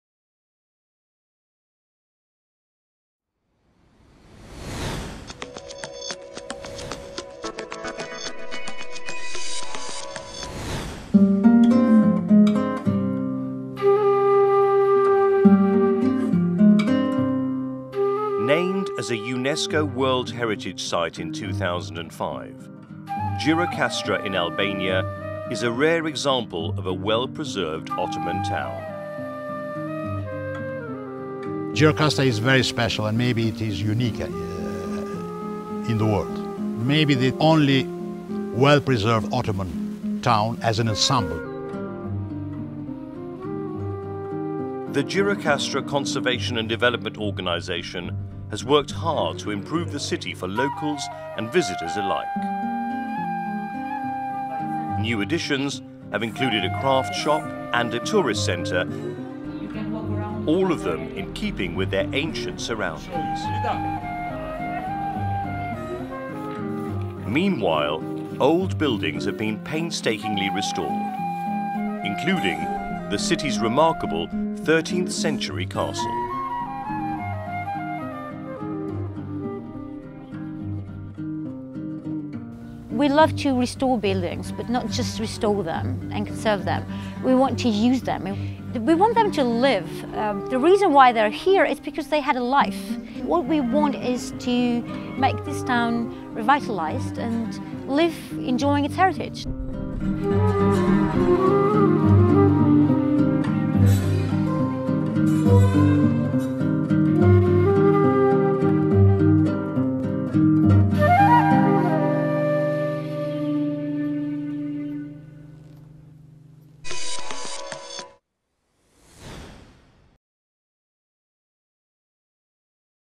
2010 Aga Khan Award for Architecture, Shortlist - Conservation of Gjirokastra, Albania
One of the 19 nominees of the 11th Cycle of The Aga Khan Award for Architecture

Location: Gjirokastra, Albania (South-East Europe)
Architect: Gjirokastra Conservation and Development Organisation, technical team
Client: Gjirokastra Conservation and Development Organisation
Completed: 2002 ongoing
Design: 2001 ongoing
Site size: 780'000 m²

The town of Gjirokastra in southern Albania is a well-preserved example of an Ottoman Balkan town, distinguished by its residential architecture but also notable for what is absent: unusually for an Ottoman town, it has only one minaret—the rest having been destroyed during the communist era. Declared a 'museum city' in 1960, Gjirokastra's architectural heritage was preserved and maintained well by its Institute of Monuments for several decades. With the collapse of the regime and the economy in 1992, however, the town's institutional structure disintegrated and skilled workers and craftsmen emigrated to Greece and elsewhere.

The Gjirokastra Conservation and Development Organisation has, for the better part of the last decade, attempted to reverse the decline of Gjirokastra's built heritage through a grassroots programme that emphasises the development potential of conservation: Preservation projects are designed with a focus on adaptive reuse and sustainability, integrating training, business development and community outreach. Its many projects to date include the restoration and reuse of the castle of Gjirokastra; the rehabilitation of the bazaar; the restoration of cobblestone streets and creation of pedestrian walkways in the old town; and the preservation of several significant buildings.